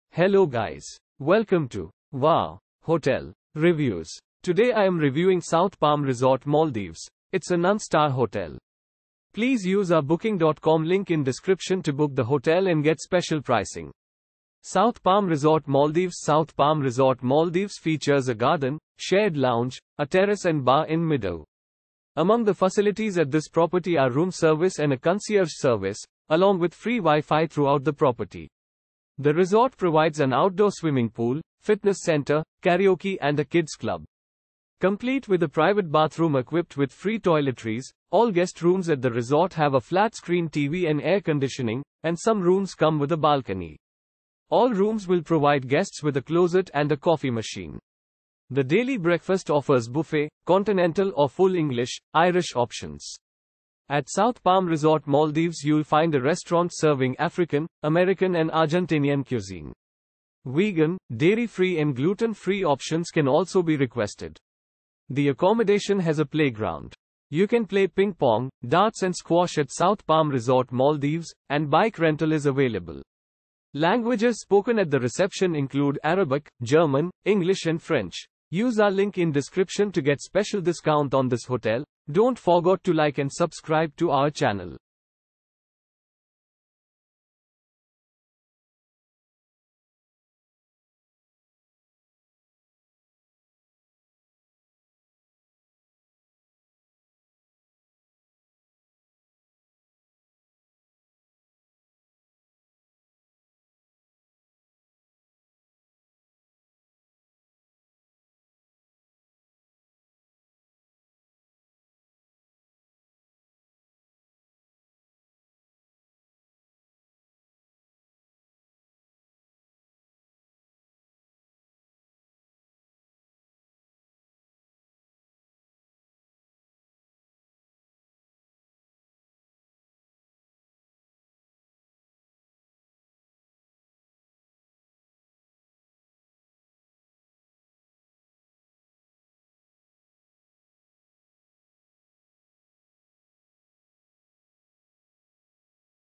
South Palm Resort Maldives Review - Midu , Maldives
South Palm Resort Maldives

South Palm Resort Maldives features a garden, shared lounge, a terrace and bar in Midu. Among the facilities at this property are room service and a concierge service, along with free WiFi throughout the property. The resort provides an outdoor swimming pool, fitness center, karaoke and a kids' club.
Complete with a private bathroom equipped with free toiletries, all guest rooms at the resort have a flat-screen TV and air conditioning, and some rooms come with a balcony. All rooms will provide guests with a closet and a coffee machine.
The daily breakfast offers buffet, continental or Full English/Irish options. At South Palm Resort Maldives you'll find a restaurant serving African, American and Argentinian cuisine. Vegan, dairy-free and gluten-free options can also be requested.
The accommodation has a playground. You can play ping pong, darts and squash at South Palm Resort Maldives, and bike rental is available.
Languages spoken at the reception include Arabic, German, English and French.